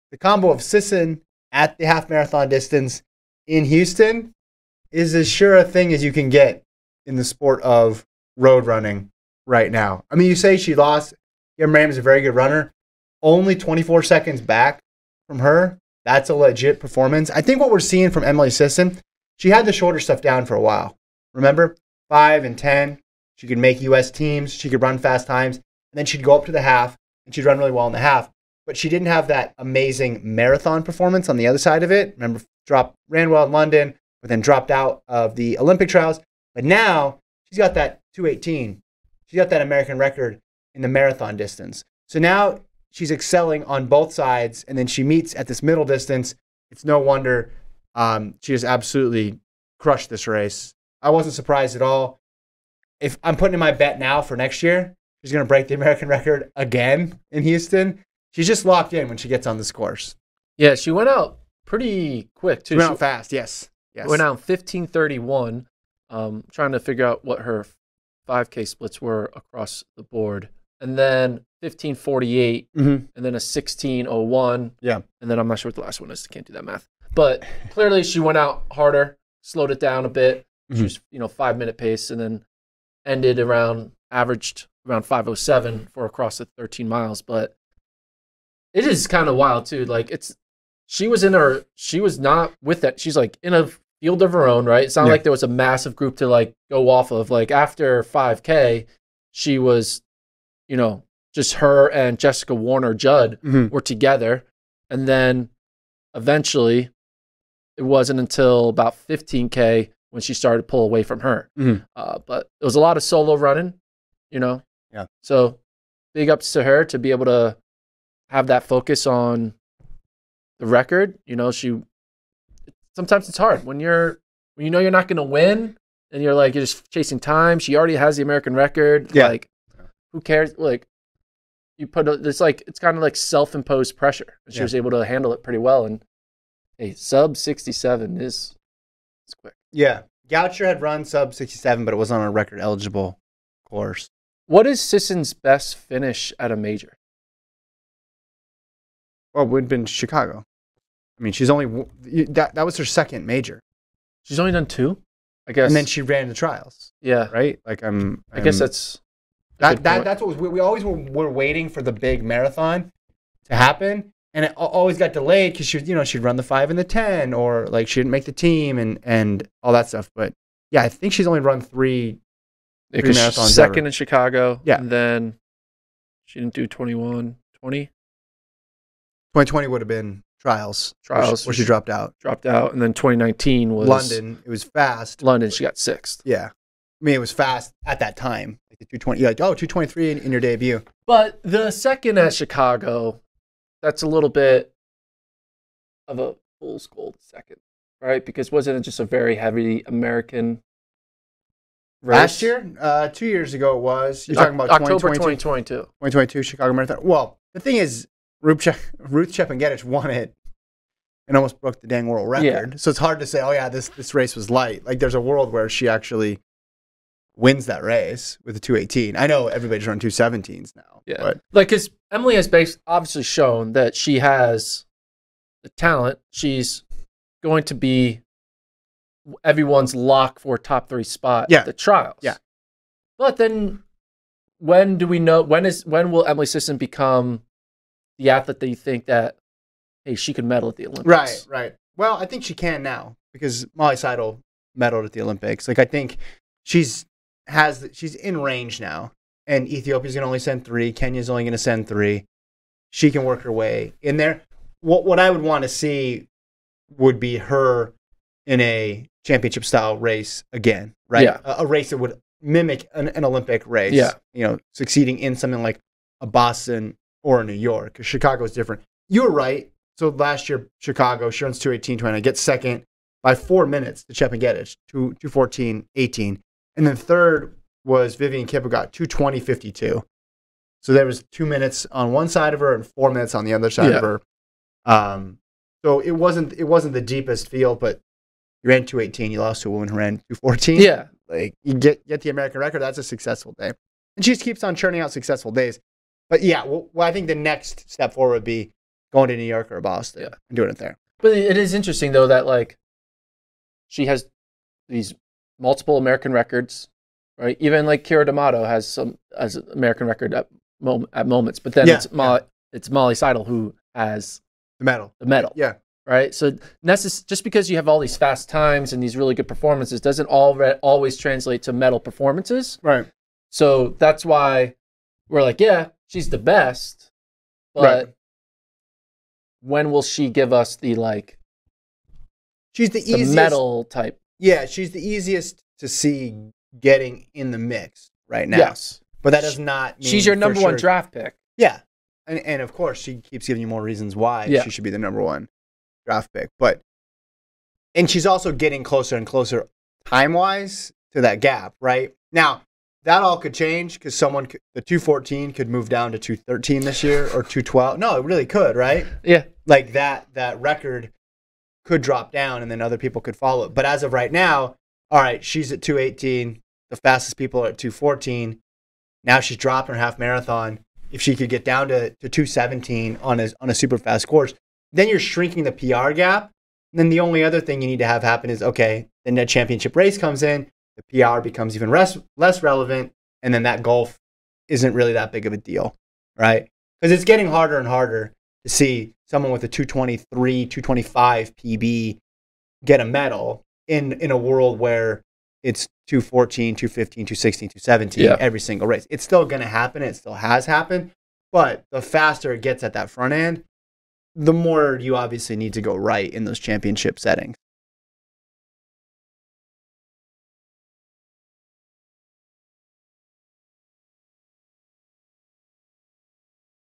Emily Sisson Breaks Her Own American Record In Half Marathon
Sisson ran 1:06:52 at the Houston Half Marathon to lower the mark she set in 2022.